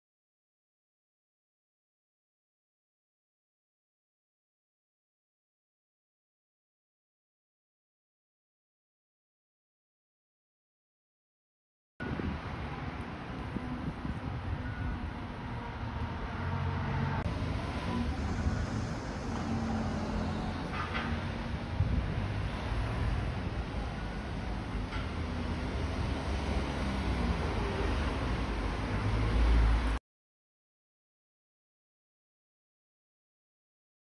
弘前上土手町10月桜開花🌸
平成3年3月24日弘前市上土手町の10月桜が開花しました♪

YouTube応援登録よろしくお願いします。